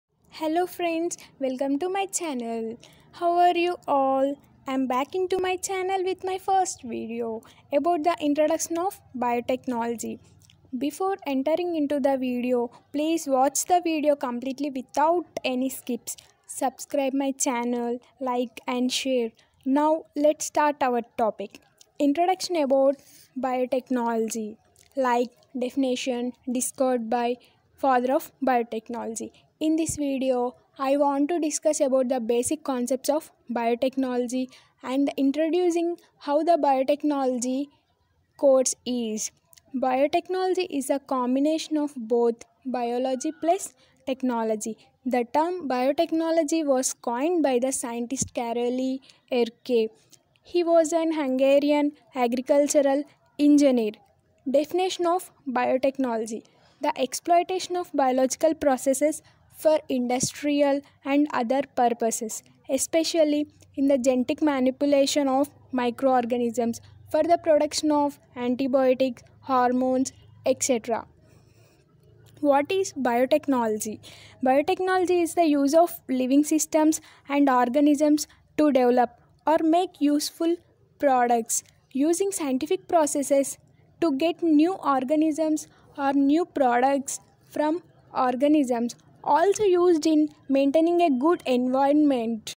Introduction about Biotechnology/Basic concepts/Father of Biotechnology/Definition of Biotechnology/
Introduction about Biotechnology course/Definition/father of Biotechnology/Basic concept with easy trick